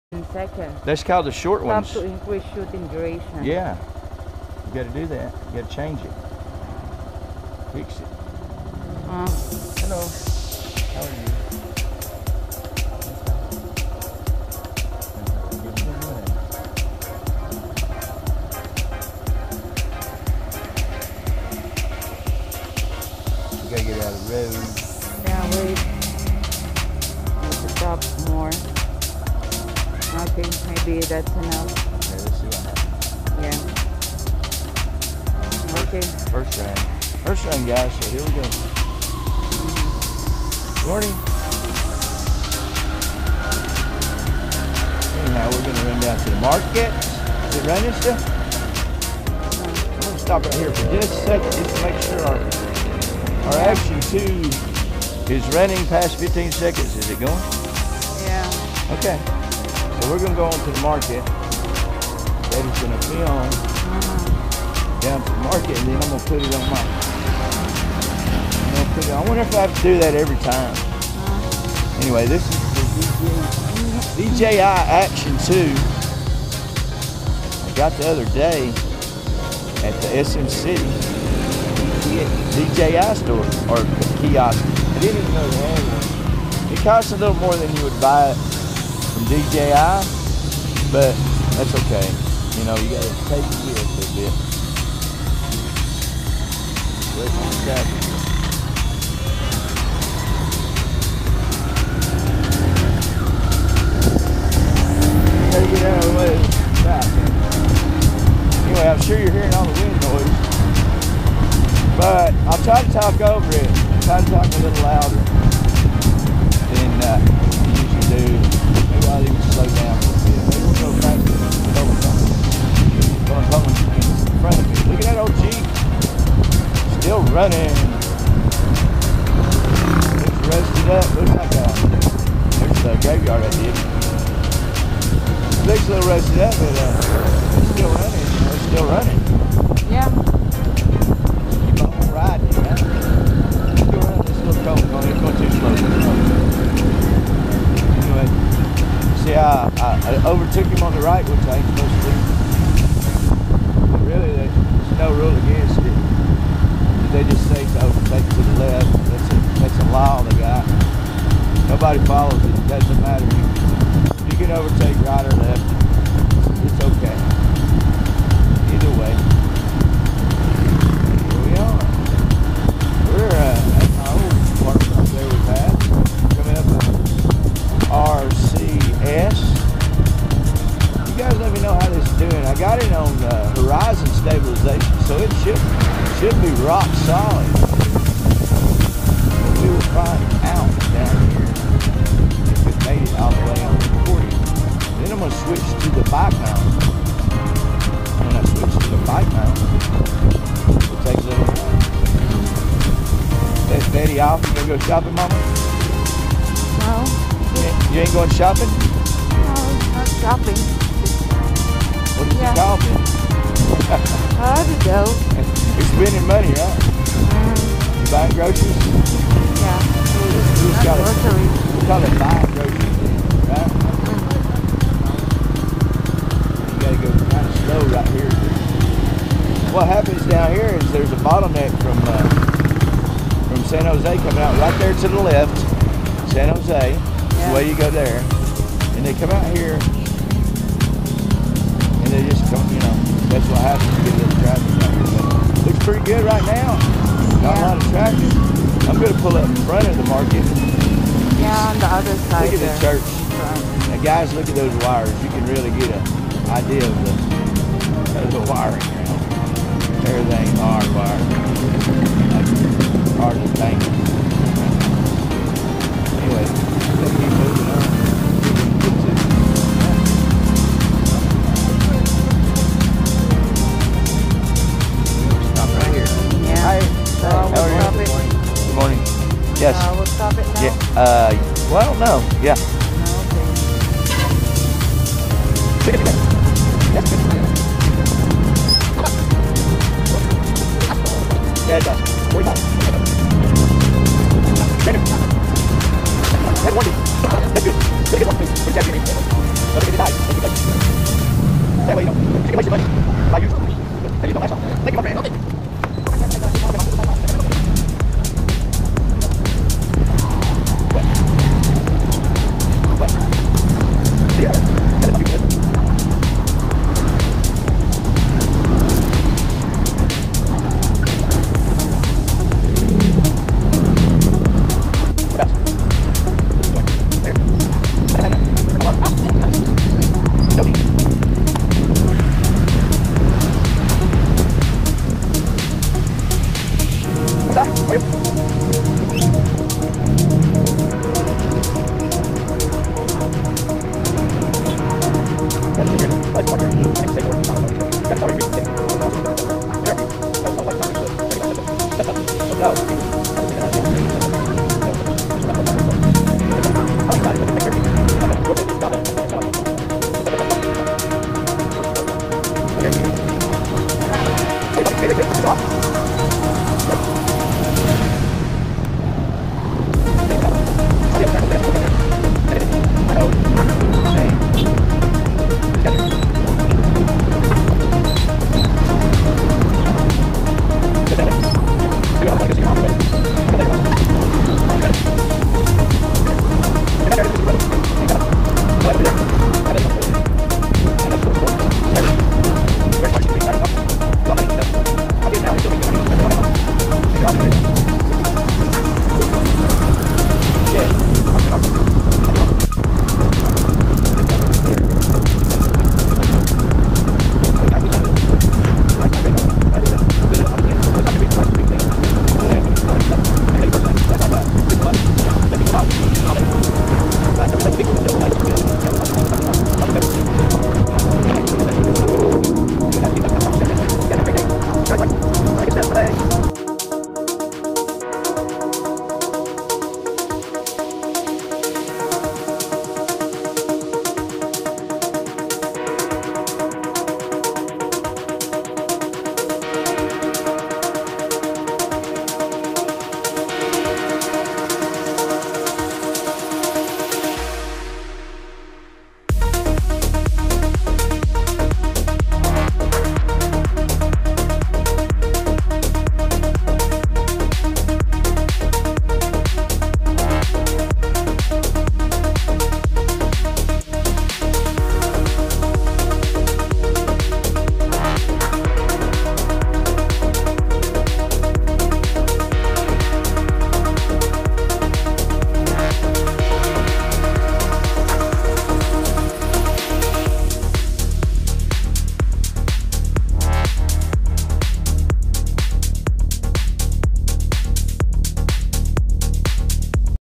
Philippines Ridin and Glidin Motorcycle & CRV
just a few clips of me and Betty on the move all about enjoy.